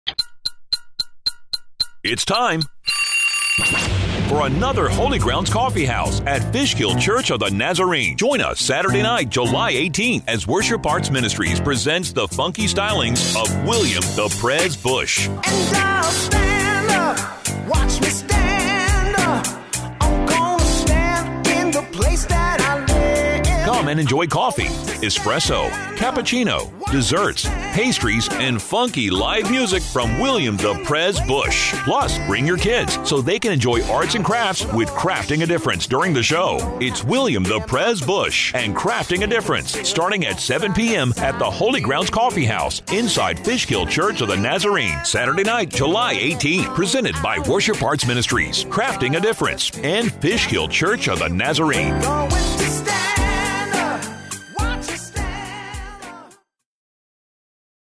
Holy Grounds Coffeehouse Presents William Bush
Come join us saturday night, July 18th for a time of food, fellowship and music by "The Funky Prez" William "The Prez" Bush!